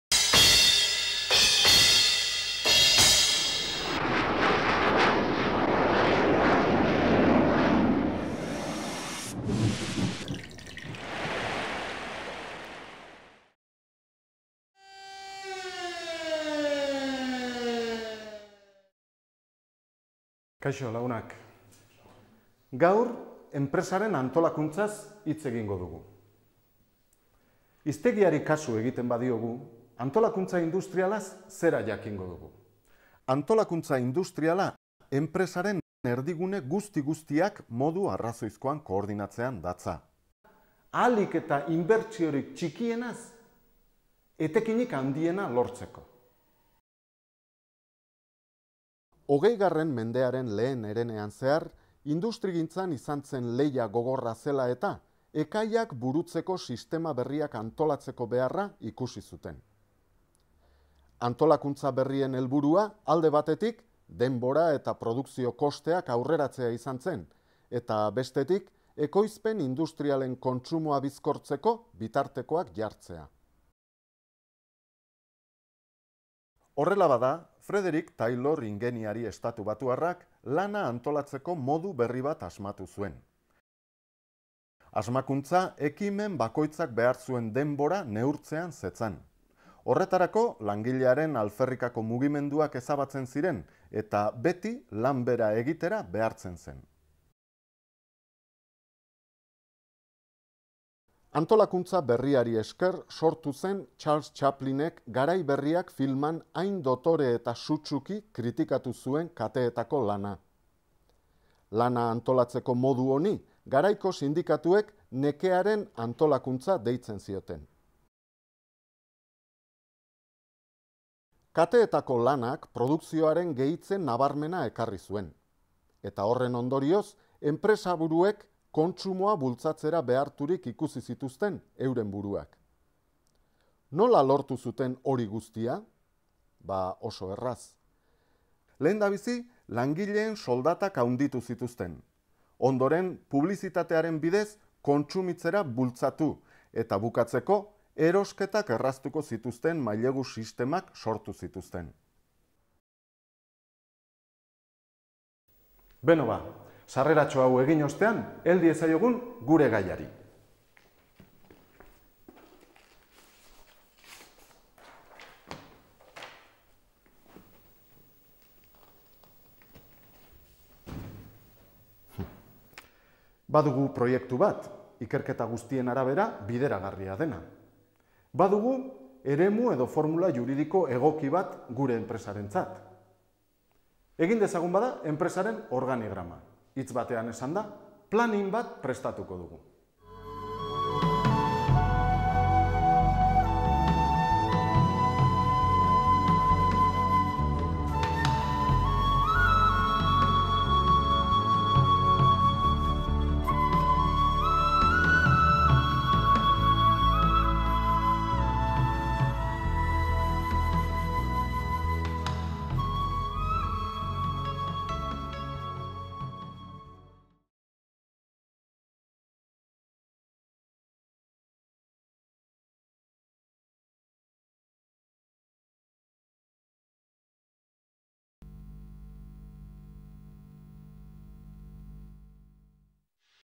ENPRESAREN MUNDUA -04- Antolakuntza: Zer da?
Ikusentzunekook ikasgai moduan ikusi nahi duenak goiko webgunean du jomuga.

Enpresaren munduak enpresaren xehetasunak erakusteko eginiko ikusentzunezko bilduma duzu. Bertan, hamabi atal bereiz daitezke, bakoitzari hainbat bideo eskaini zaizkiolarik.

Irakasmaila: Batxilergoa
Jakintzagaia: Ekonomia eta enpresa
Irakasteredua: D